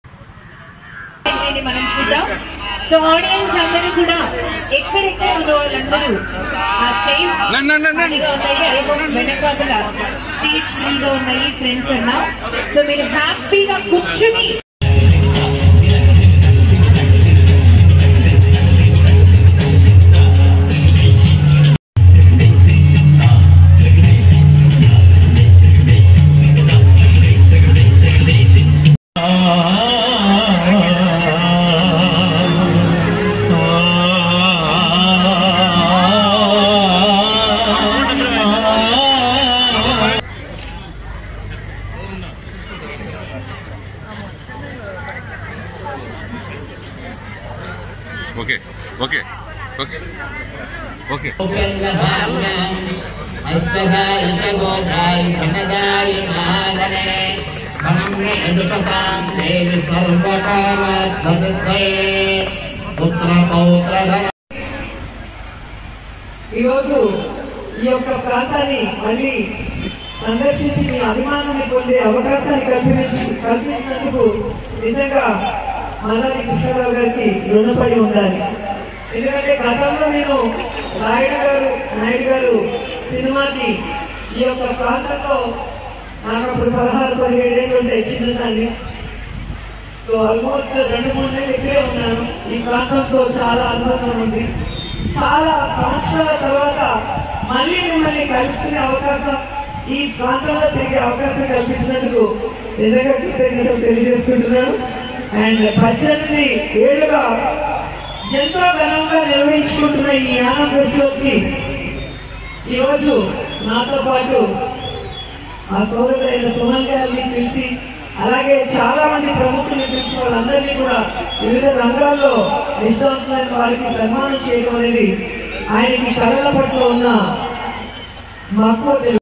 YSRCP MLA RK Roja participates in Yanam cultural programs
YSRCP MLA RK Roja participates in Yanam cultural programs - 7th Jan 2020